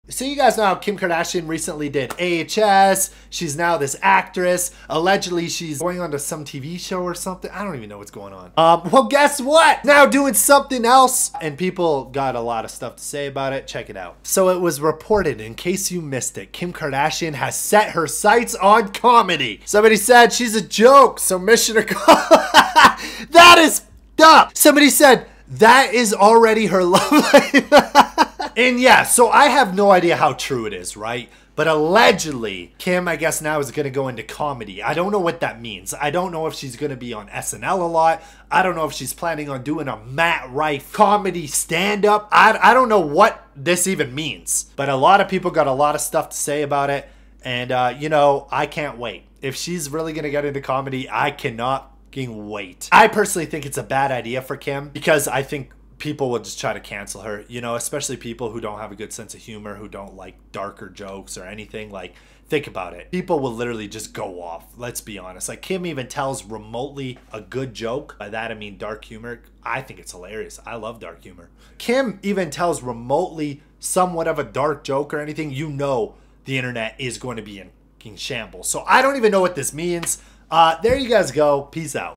Kim Kardashian Finally DECIDES WHAT NOW!!?!?! | She's TAKING OVER
KUWTK star Kim Kardashian is officially TAKING OVER!!! | she was on American Horror Story, did acting and NOW doing COMEDY??? | is this like Pete Davidson SNL or stand up??? don't even know what this means